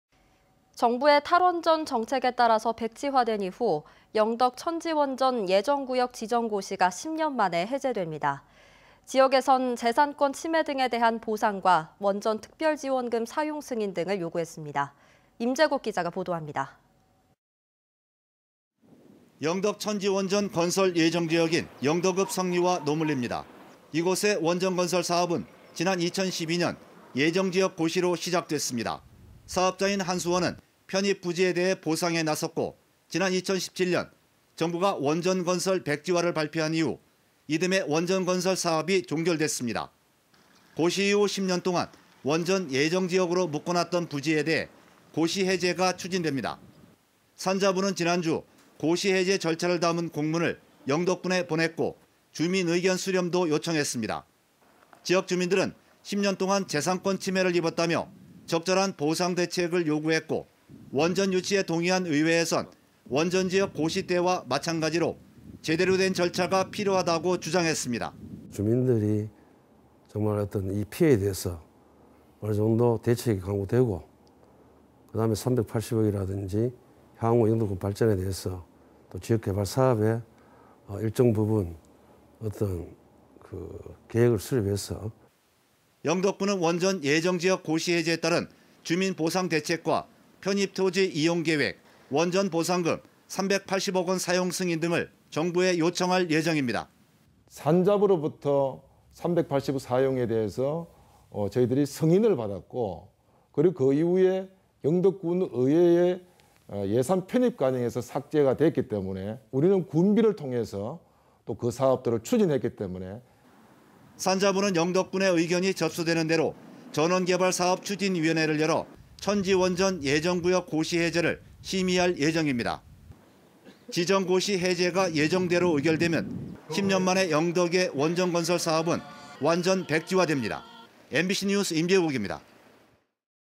영덕 천지원전 예정 고시 10년만에 해제 (2021.02.22/뉴스데스크/포항MBC)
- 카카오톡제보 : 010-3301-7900
- 카카오톡ID : PHMBCJEBO